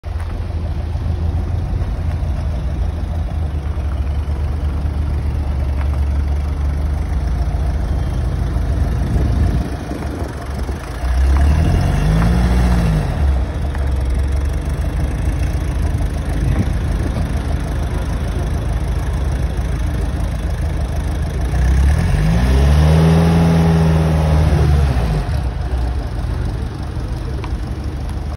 Porsche 912 E Spotted porsche #912
Lovely looking Porsche 912 E spotted at Brooklands Museum German day October 2023.